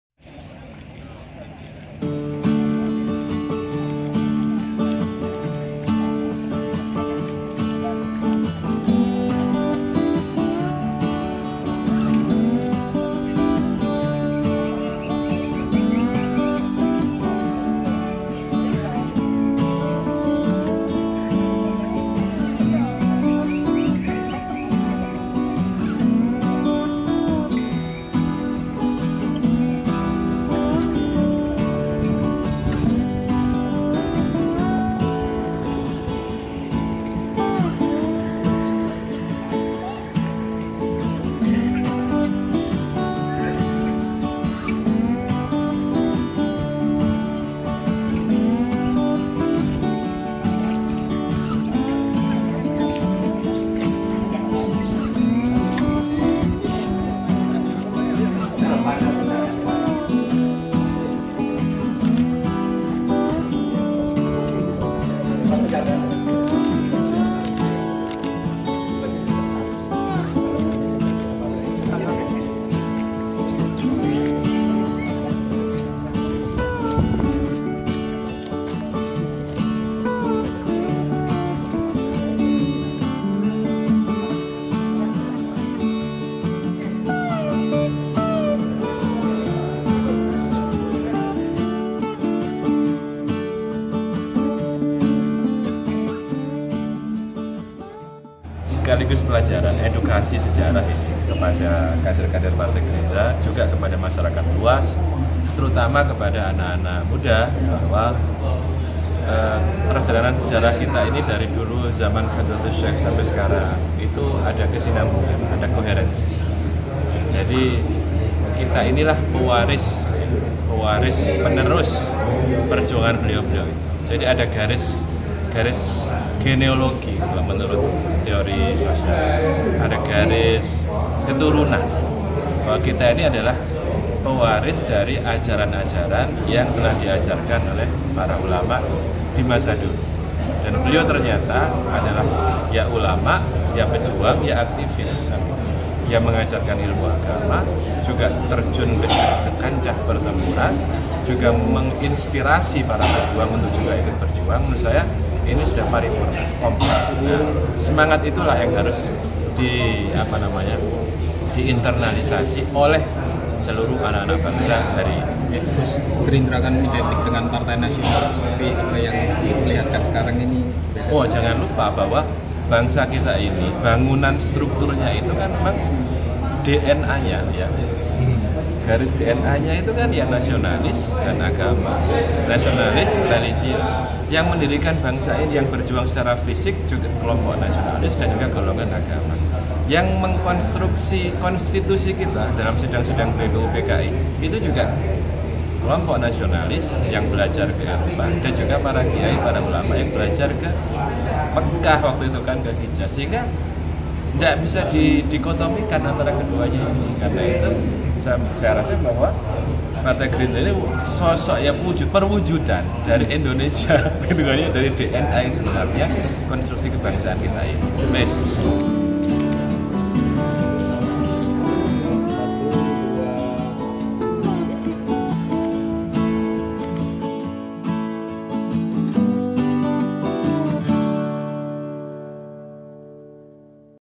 Gerindra Jatim Napak Tilas Perjuangan Pendiri NU
jatimnow.com - Pengurus Gerindra Jawa Timur (Jatim) memaknai Hari Santri tahun 2022 dengan menapaktilasi perjuangan pendiri Nahdlatul Ulama (NU) Hadrattusyaikh Hasyim Asy'ari di Surabaya.

Ketua Gerindra Jatim Anwar Sadad terlihat memimpin napak tilas itu. Tampak pula beberapa pengurus partai, DPC Gerindra Surabaya, hingga anggota DPRD Jatim dari Fraksi Gerindra.